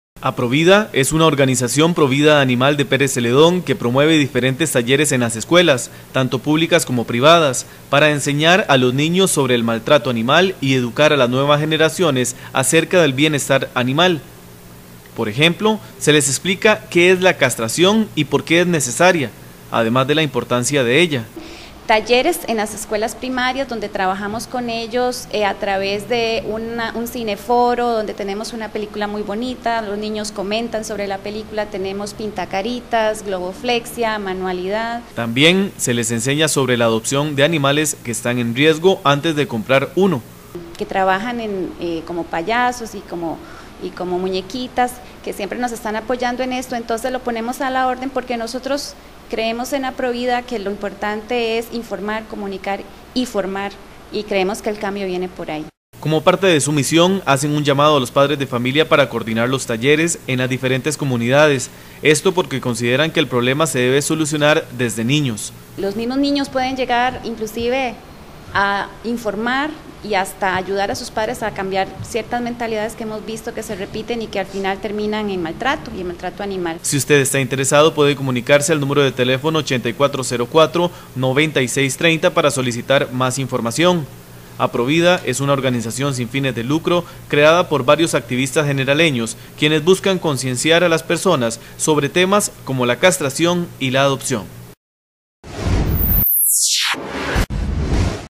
Inscriba a su Escuela en los talleres sobre el cuidado y bienestar animal
Aprovida, es una Agrupación Pro Vida Animal de Pérez Zeledón, que promueve diferentes talleres en las escuelas, tanto públicas como privadas, para enseñar a los niños sobre el maltrato animal y educar a las nuevas generaciones acerca del bienestar animal.